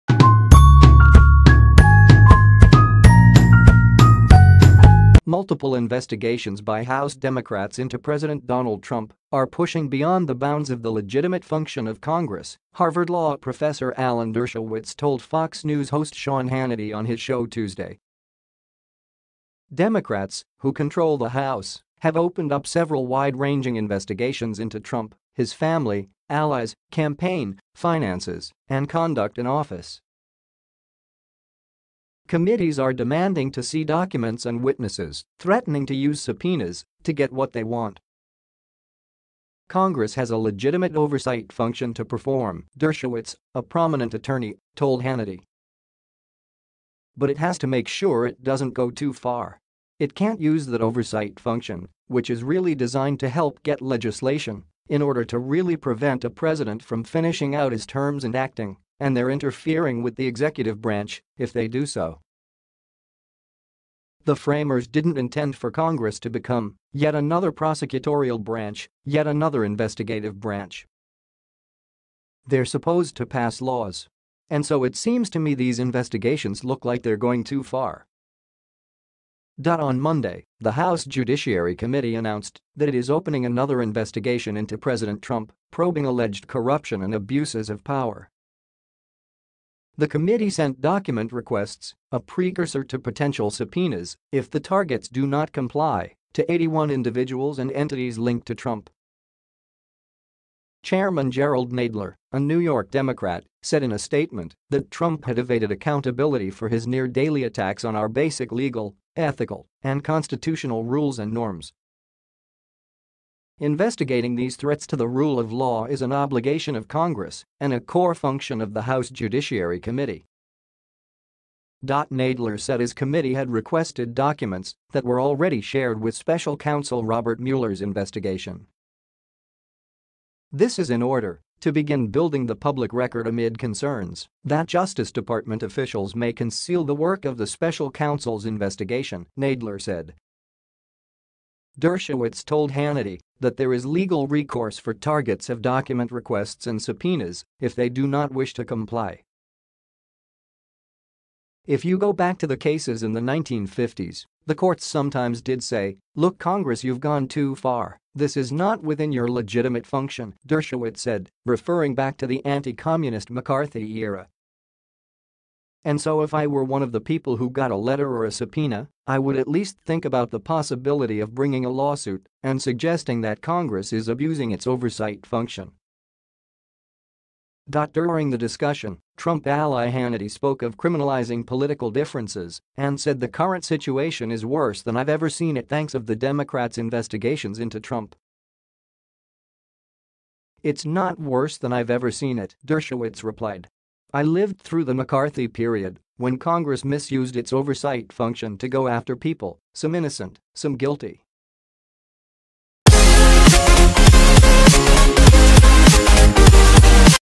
Multiple investigations into Trump are pushing beyond the bounds of the “legitimate of Congress"
Multiple investigations by House Democrats into President Donald Trump are pushing beyond the bounds of the “legitimate function of Congress,” Harvard